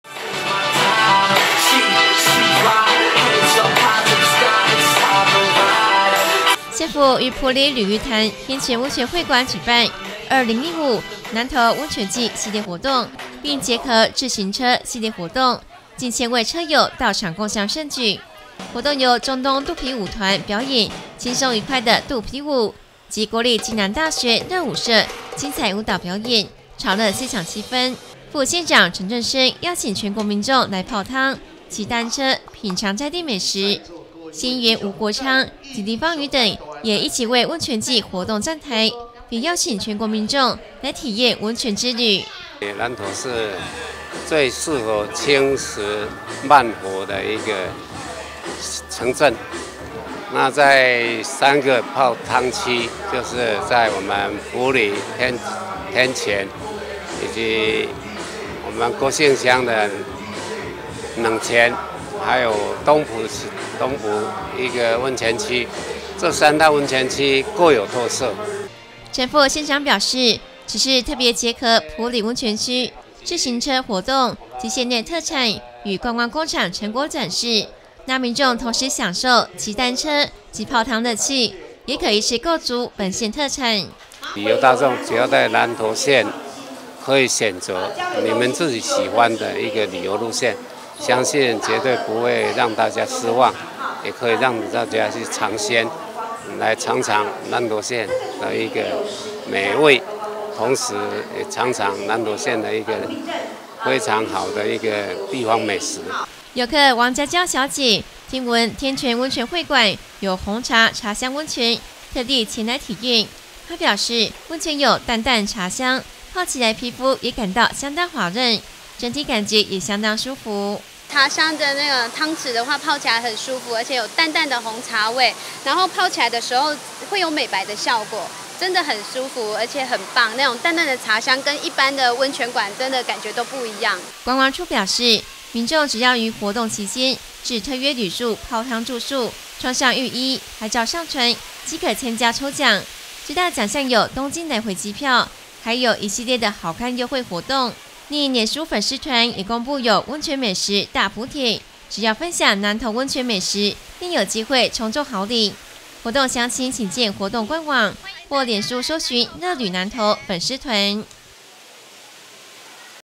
2015南投溫泉季 邀請全國民眾來泡湯
喜愛泡湯的民眾，南投溫泉季來嘍！31日埔里溫泉季率先起跑，縣府於埔里鯉魚潭天泉溫泉會館舉辦2015南投溫泉季系列活動--「i慢玩埔里溫泉知性之旅」，並結合自行車系列活動，近千位車友到場共襄盛舉。副縣長陳正昇邀請全國民眾來泡溫泉、騎單車、品嚐在地美食及感受各產業特色。